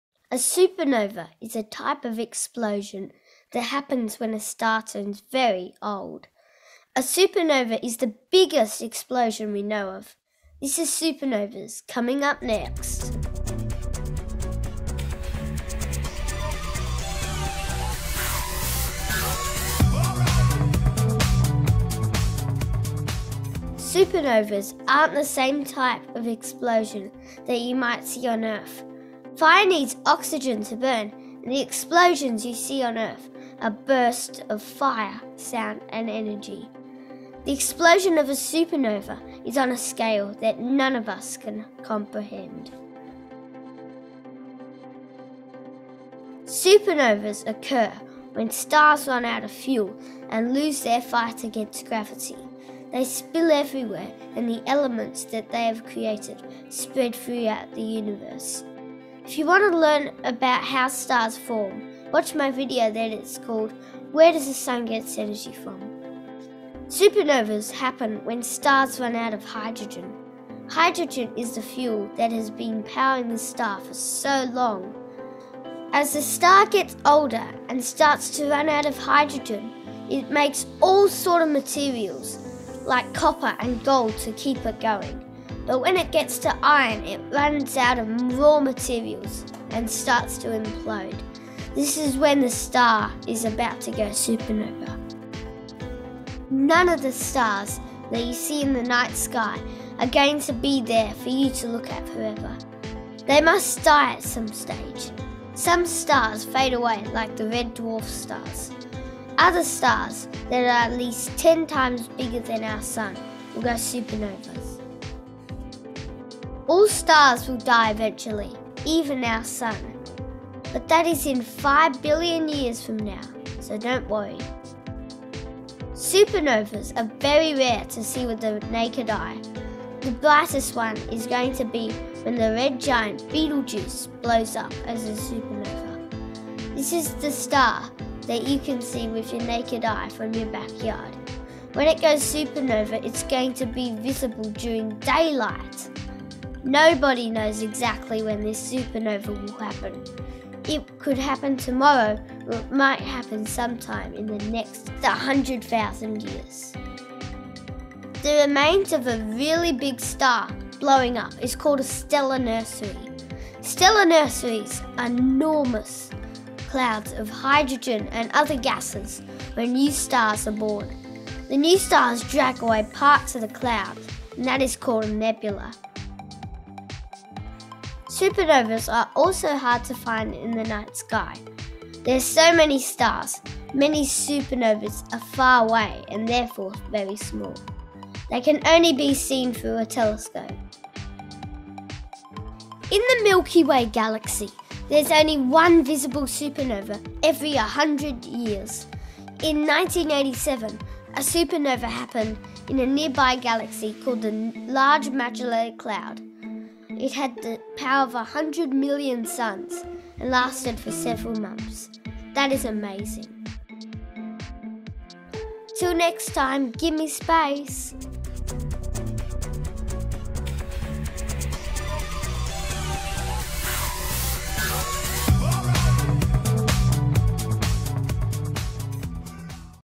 Supernovas: The biggest explosion in the universe
Learn about supernovas, the biggest explosion in the universe. Supernovas happen when stars run out of fuel and collapse under their own gravity. Supernovas have created most of the elements that make up the earth.